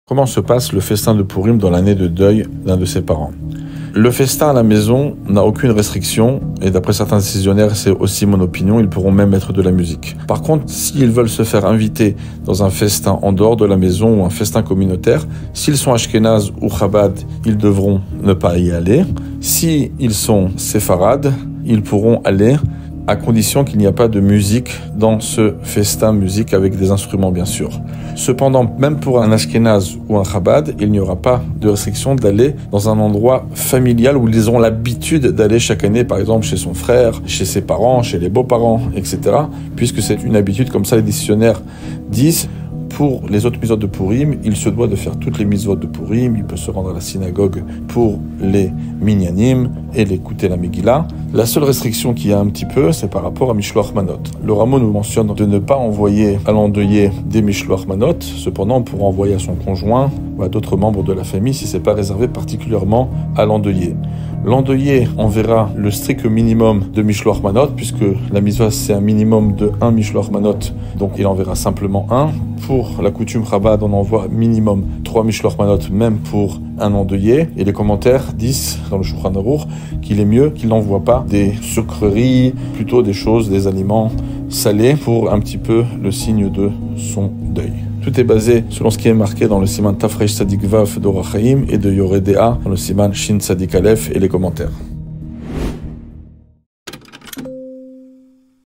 Les Mitsvoth de Pourim une année de deuil | Rav Mikhael Abichid
🎙 Dayan - Rav Mikhael Abichid

🎬 Comment accomplir les Mitsvoth de Pourim durant une année de deuil ?

Centre Habad Loubavitch Francophone d'Erets Israël (depuis 1973)

Facebook : Habad Loubavitch Francophone d'Erets Israël